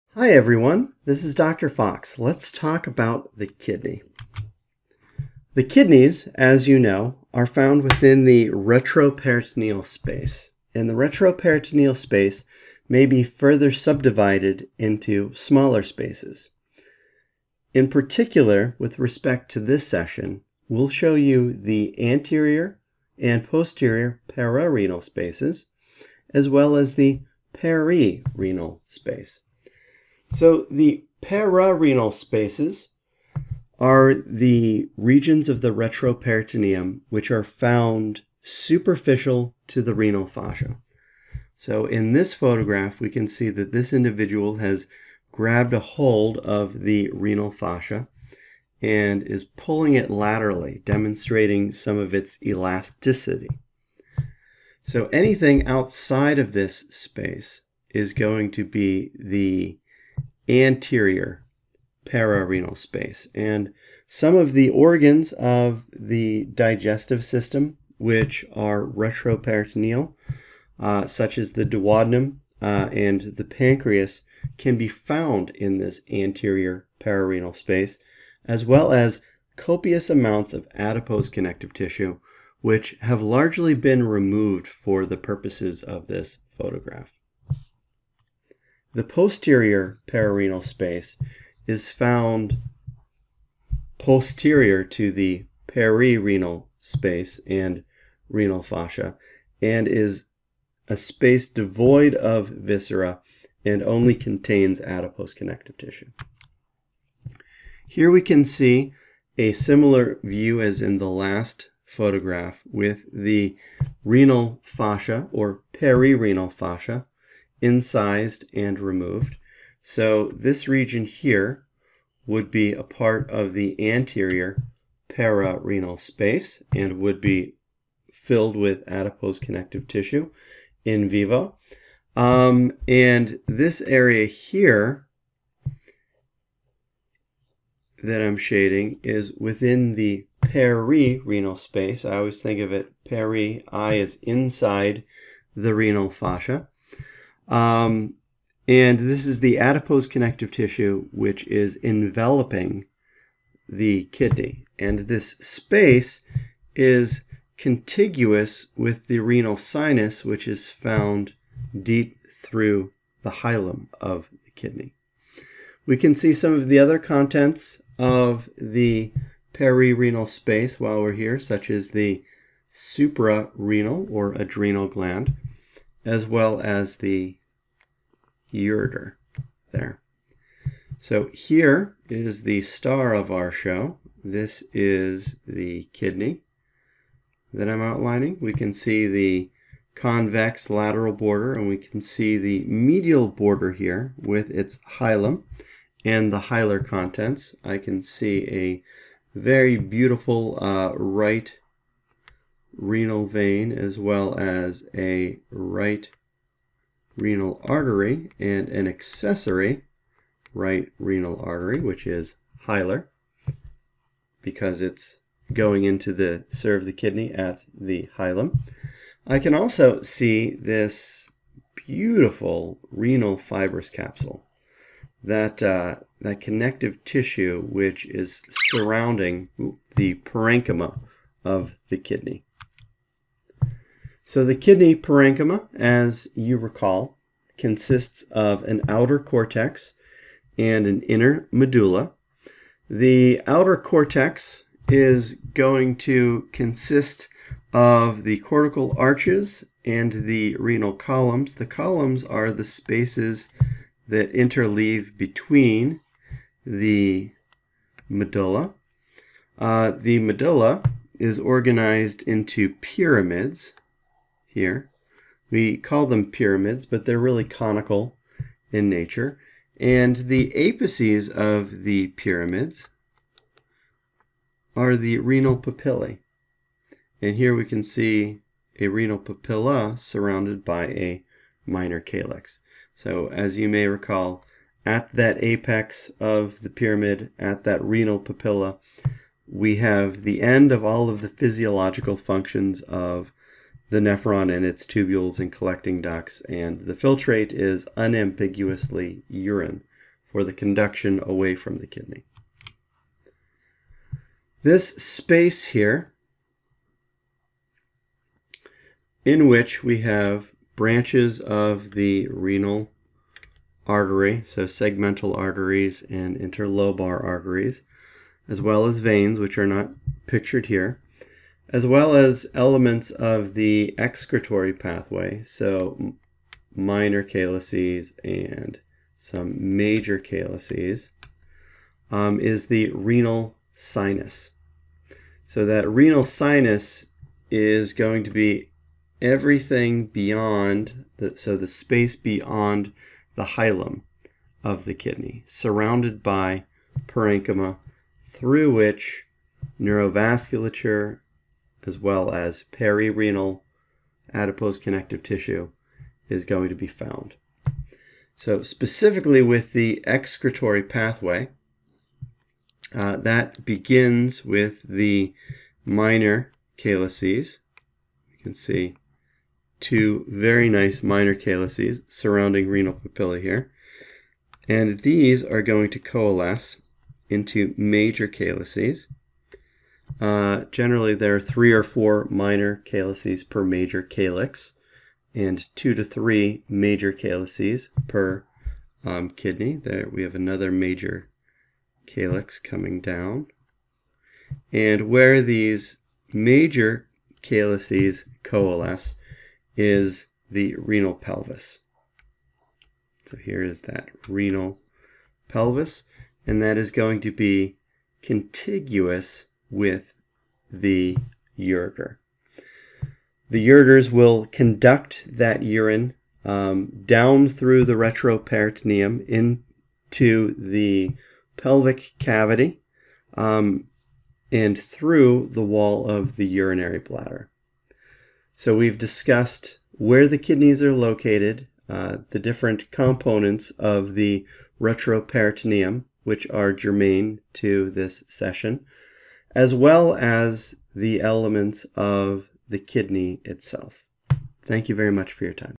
Kidneys and Urinary System Dissection Supplement - Kidney
Dr. Glenn Fox discusses how to identify gross structures of the kidney in lab, as part of the Supplemental Dissection Videos series for the First Year Medical Anatomy course at the University of Michigan Medical School.